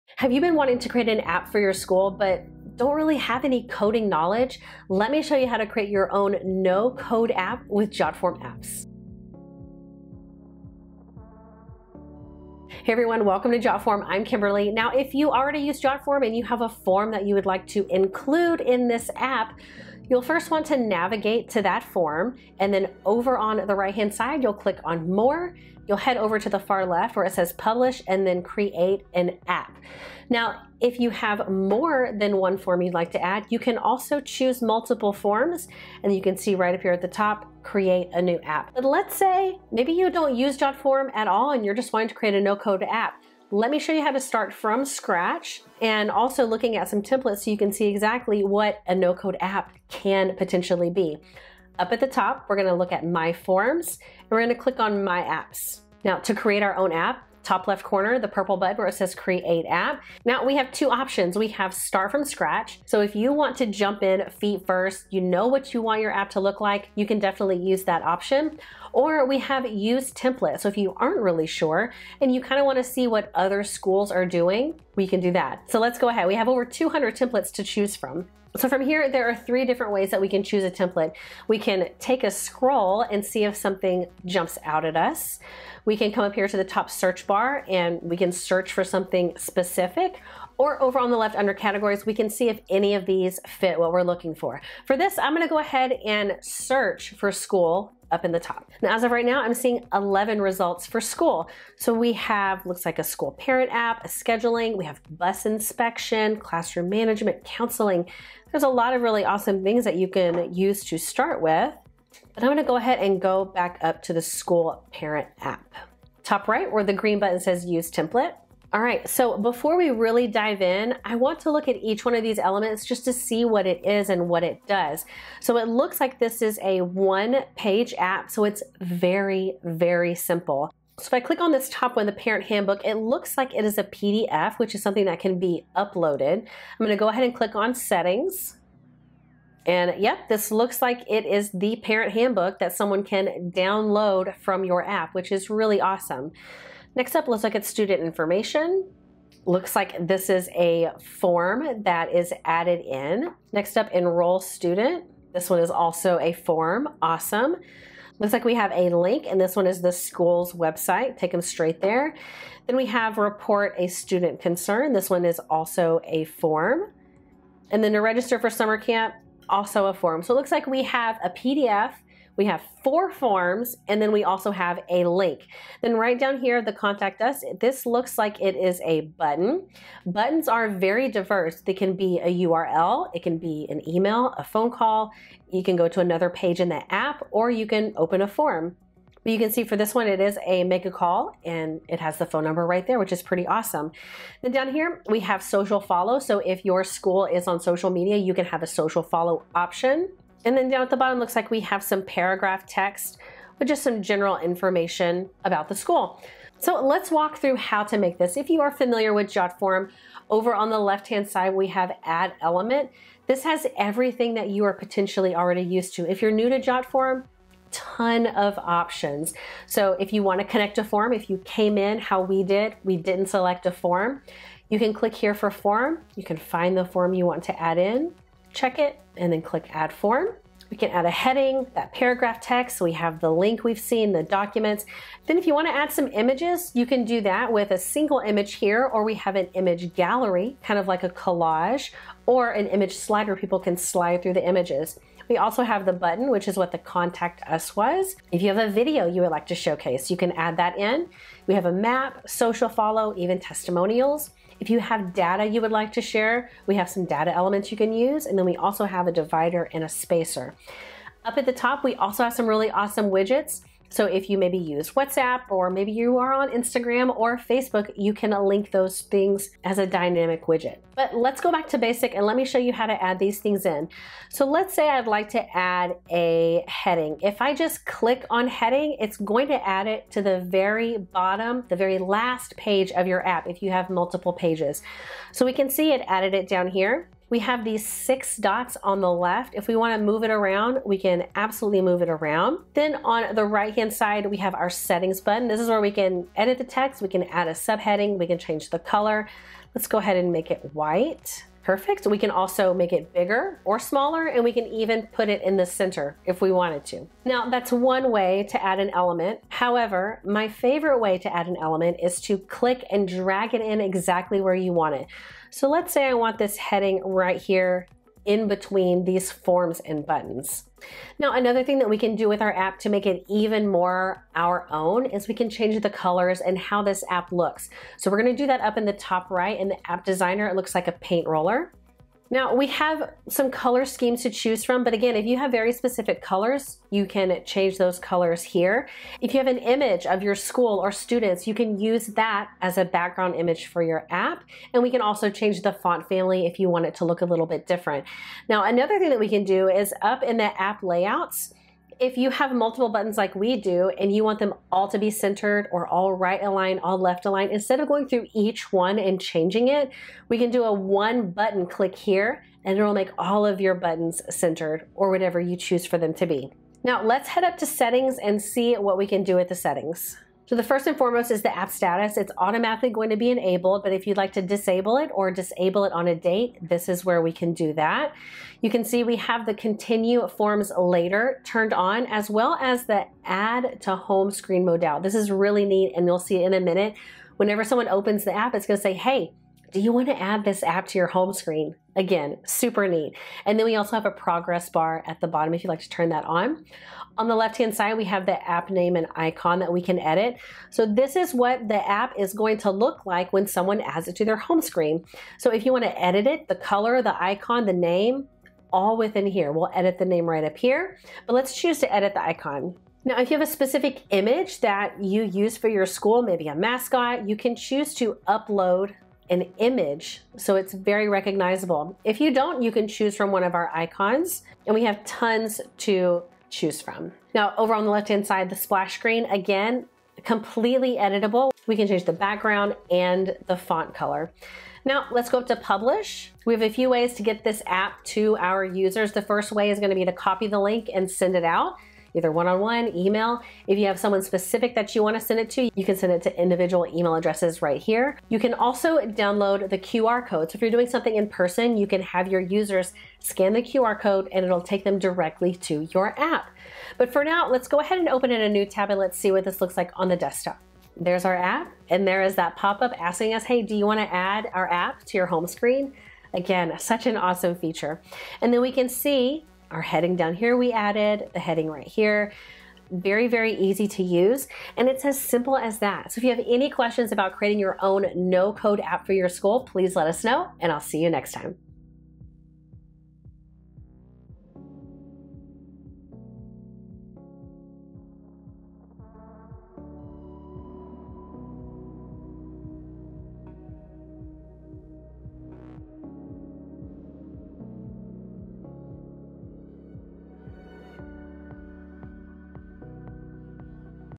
Create a No-Code App for Your School
Whether you need to collect student registrations, waivers, or share important information, create a free app for your school with Jotform Apps! No coding knowledge needed — just drag and drop to design the perfect app for your needs.
LINKS & RESOURCES
👋 ABOUT JOTFORM
Hi, we're Jotform, a full-featured online forms platform that makes it easy to create robust forms and collect important data. Check us out
00:00 Introduction
00:12 Create an App
01:09 App Templates
01:58 App Elements
04:21 Widgets
04:32 Add Elements to App
04:52 Element Settings
05:10 Drag & Drop Feature
05:20 App Design
06:06 App Settings
07:15 Publish the App
08:02 Subscribe to Jotform